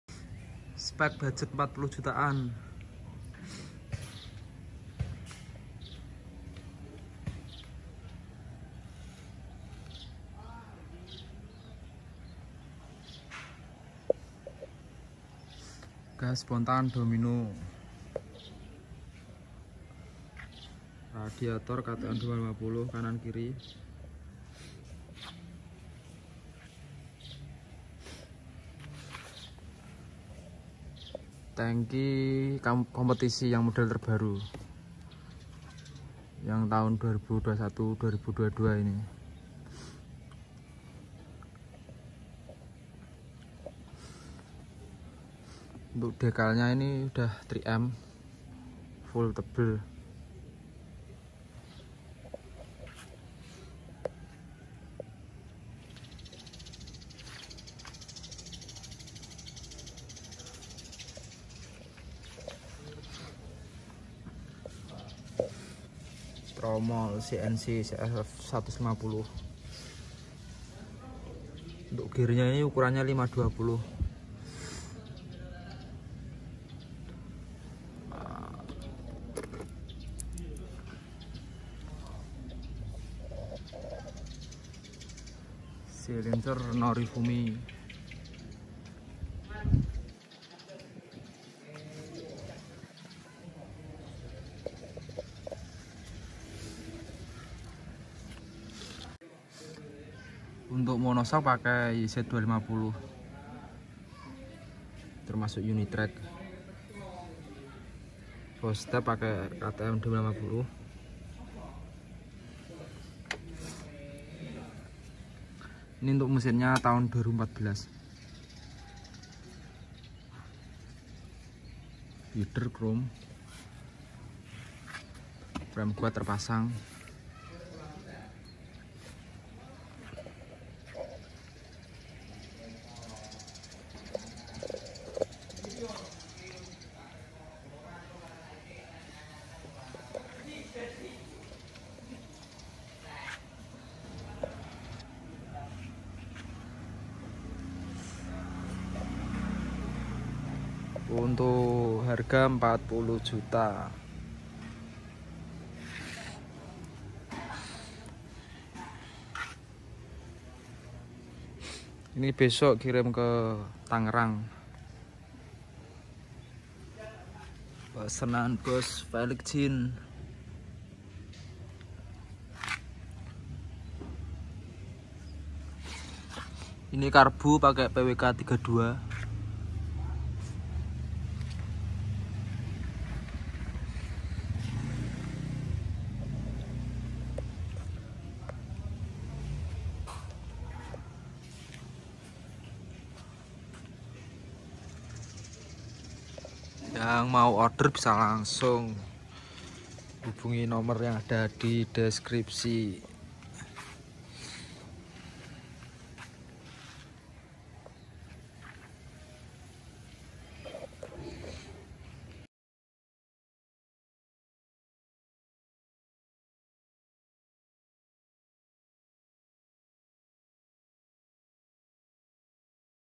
Kawasaki ninja rolling ktm 2021 murah,ninja trail review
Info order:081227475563
Alamat Bengkel:
Kec.cepu
Kab.blora
Desa ngelo jalan order baru Belakang Hotel atrindia

Untuk divideo ini biaya 40jt an( mesin termasuk dari kita)
Basic mesin tahun 2014

Video proses perakitan klik

Terima kasih